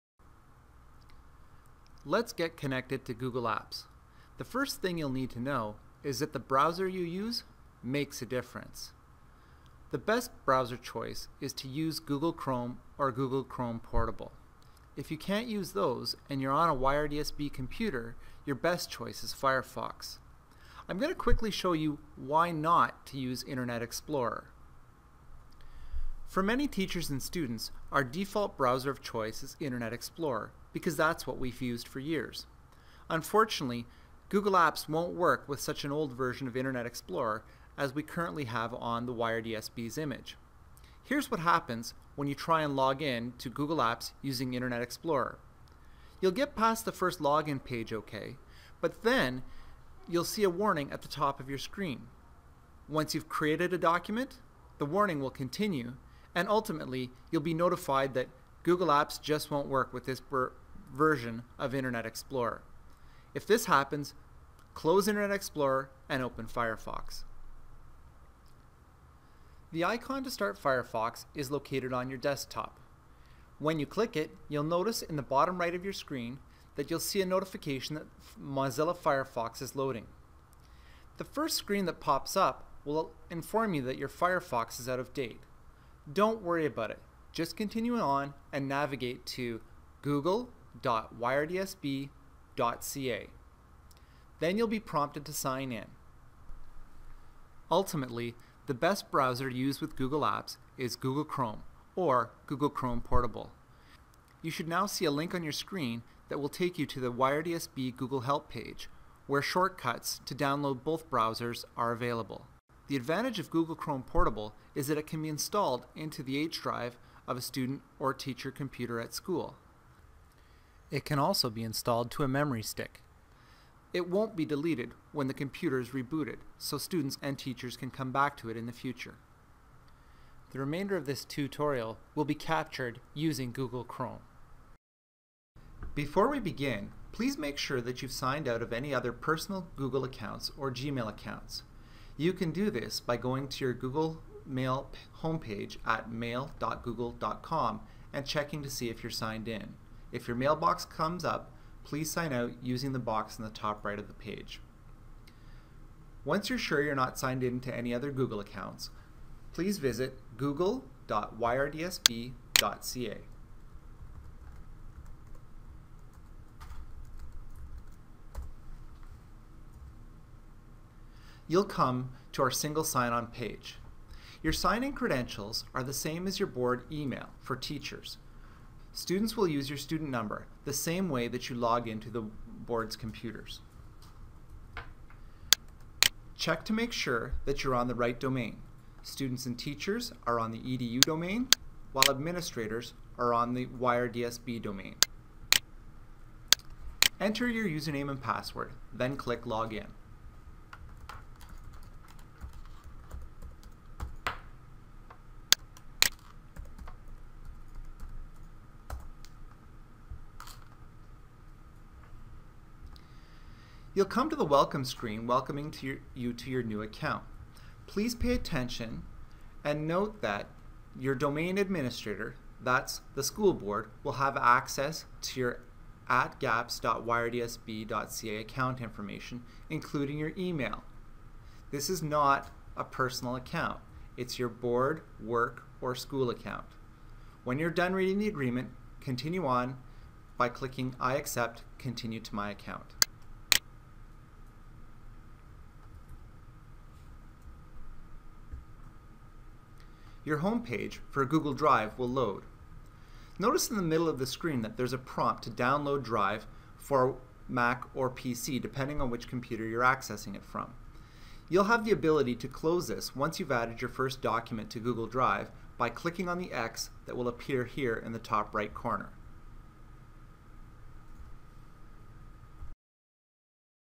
Getting Connected to Google Apps in the YRDSB (***See May 2017 update below)
Update: May 22, 2017 - I'm aware of the "An error occurred and single sign-on could not complete. Please close your browser and try again." message users are getting. There appears to be a problem with our YRDSB Single Sign On Page. It has been reported to IT Services and escalated for a response. I'm no longer a DLRT, so not necessarily getting the most up to date information, but if I hear anything else I'll share here.

Learn how to access your YRDSB Google Apps account for the first time.

NOTE:  DO NOT TRY USE THE VERSION OF MICROSOFT INTERNET EXPLORER INSTALLED ON YRDSB WINDOWS XP COMPUTERS.  IT IS OUT OF DATE.  USE MOZILLA FIREFOX INSTEAD.

You'll need:

1.  A modern browser.  Google Chrome works best and links to both the regular version and portable version can be found on our YRDSB Google Help

2.  If you have a personal Gmail (or other Google account), you should log out of it first, unless you're comfortable working with multiple sign ons.

3.  If you're working on a YRDSB computer, use Firefox (found on the desktop).  If you can't get Firefox running, download and install Google Chrome Portable to a memory stick (best) or your H:\\ drive (My Documents)

4.  The URL to access Google Apps for the YRDSB is

5.  Your username and password are the same as you would use to log on to a YRDSB Windows computer.